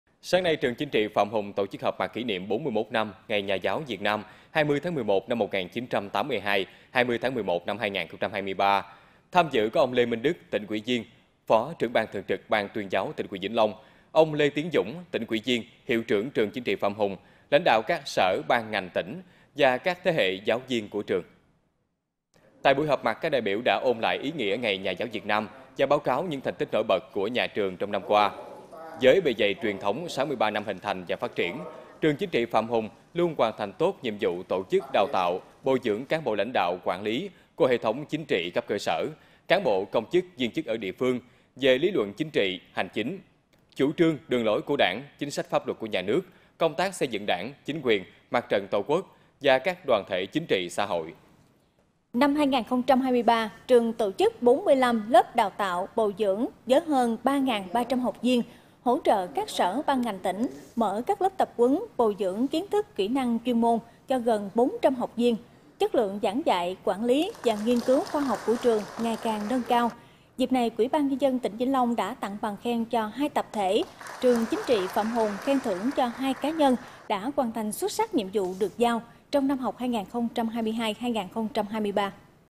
Trường chính trị Phạm Hùng họp mặt kỷ niệm 41 năm ngày Nhà giáo Việt Nam
Tại buổi họp mặt, các đại biểu đã ôn lại ý nghĩa ngày Nhà giáo Việt Nam và báo cáo những thành tích nổi bật của trường trong năm qua. Với bề dày truyền thống 63 năm hình thành và phát triển, Trường Chính trị Phạm Hùng luôn hoàn thành tốt nhiệm vụ tổ chức đào tạo, bồi dưỡng cán bộ lãnh đạo quản lý của hệ thống chính trị cấp cơ sở, cán bộ công chức viên chức ở địa phương về lý luận chính trị-hành chính; chủ trương đường lối của Đảng, chính sách pháp luật của nhà nước; công tác xây dựng Đảng, chính quyền, mặt trận tổ quốc và các đoàn thể chính trị- xã hội.

⬇️⬇️⬇️ Tải THVL Audio để nghe đầy đủ